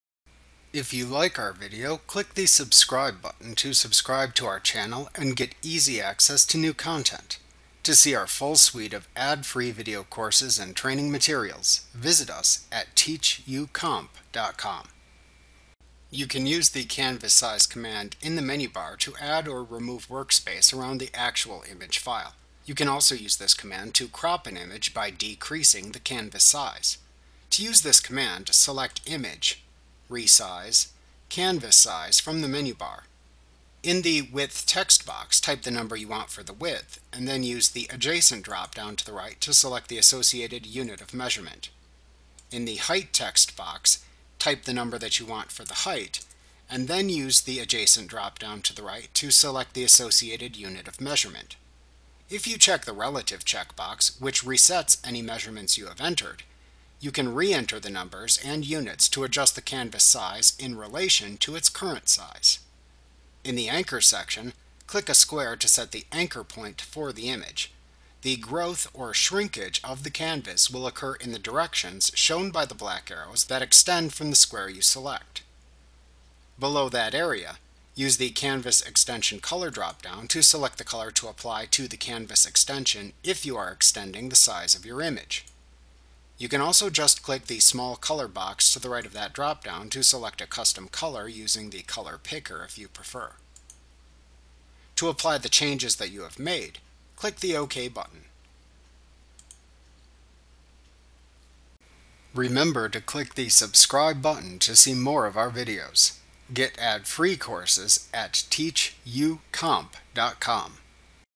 Photoshop Elements 2021 Tutorial Changing the Canvas Size Adobe Training
Learn about Changing the Canvas Size in Adobe Photoshop Elements 2021 with the complete ad-free training course